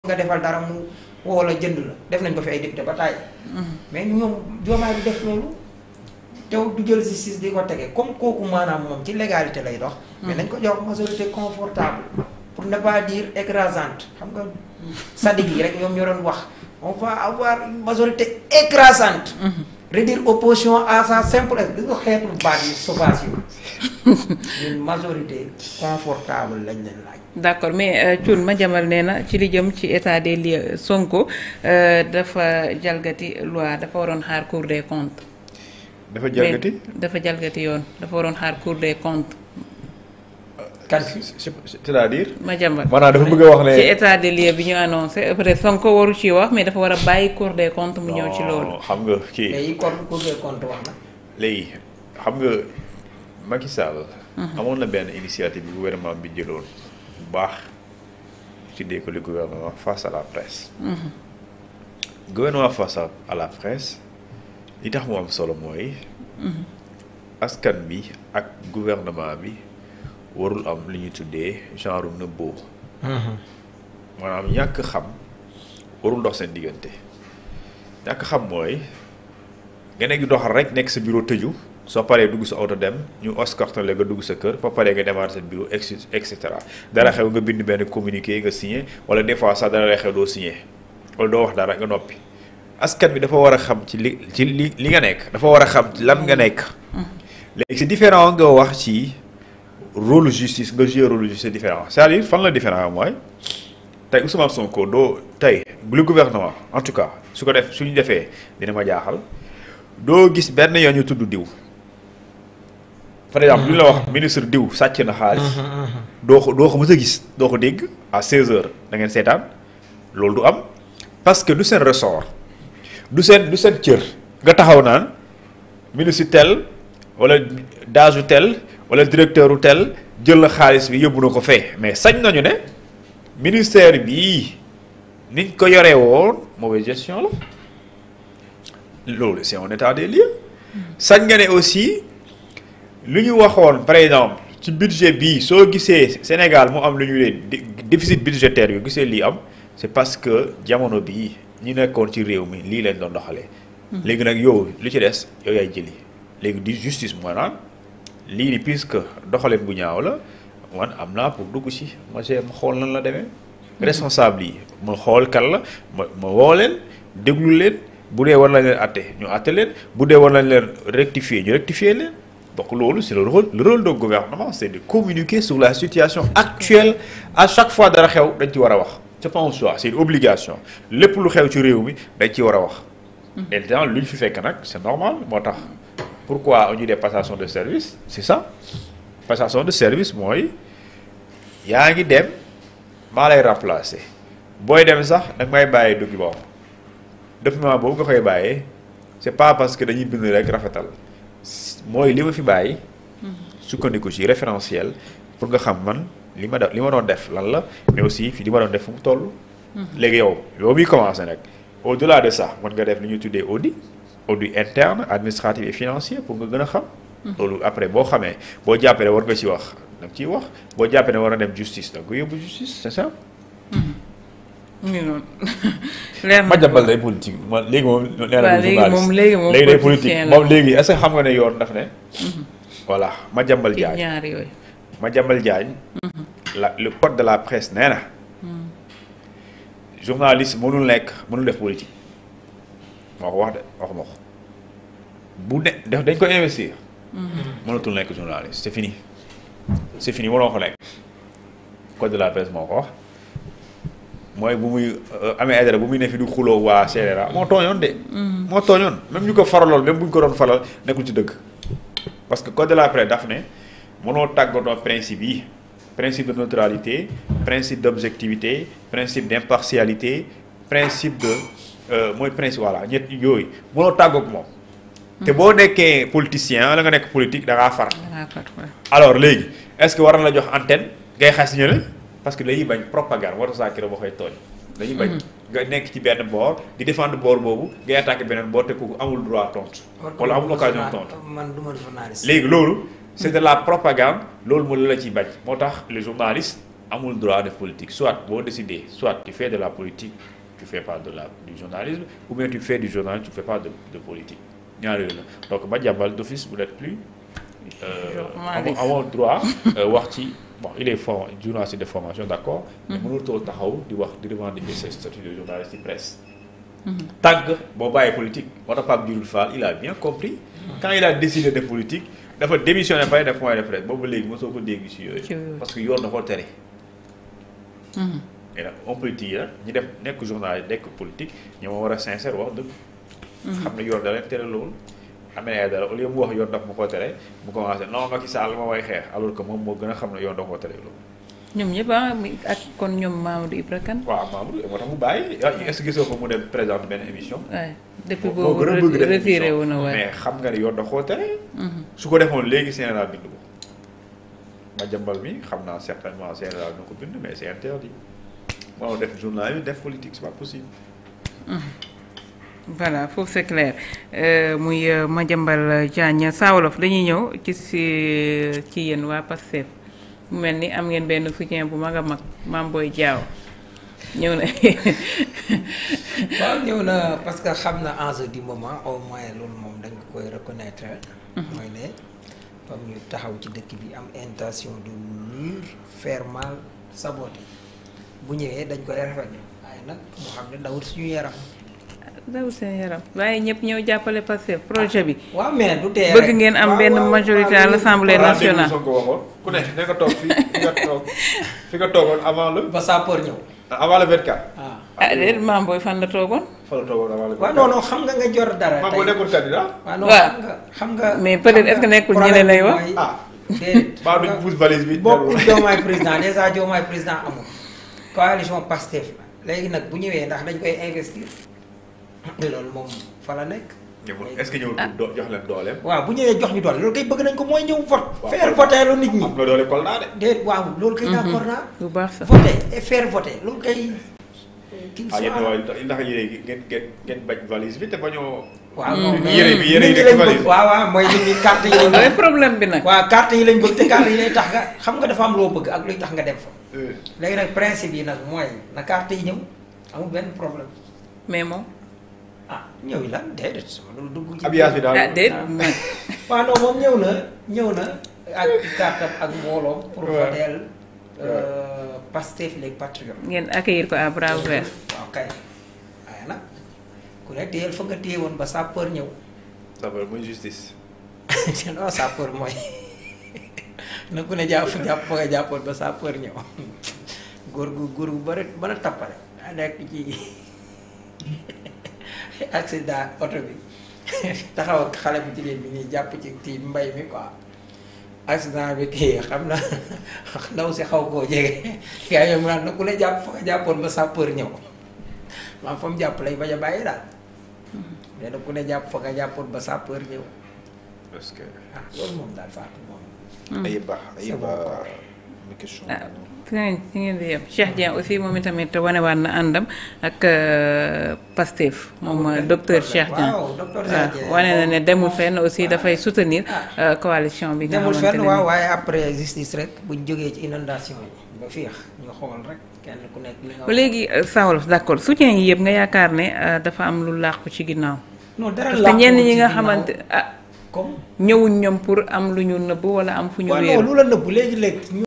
Alerte🛑Le juriste Thioune démasque Madiambale en direct "li yoone wakh moy meno nek... "
Urgent🛑Sonko en direct réagit- arrestation de Khadim Bâ Locafrique-  Mame Boye Diao avec Pastef- Madiambal clash